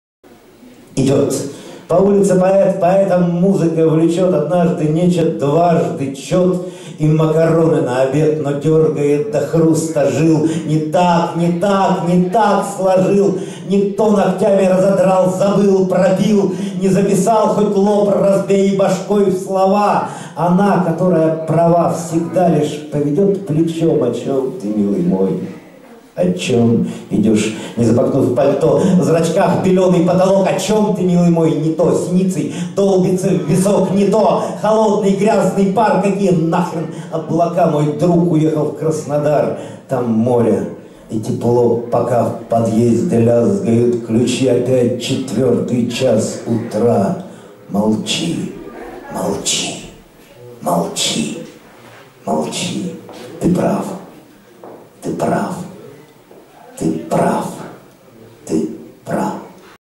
48 Идет по улице поэт...- Андрей Мансветов
Стихи "Идет по улице поэт..." читает Андрей Мансветов на Открытии сезона в ДА (Ижевск) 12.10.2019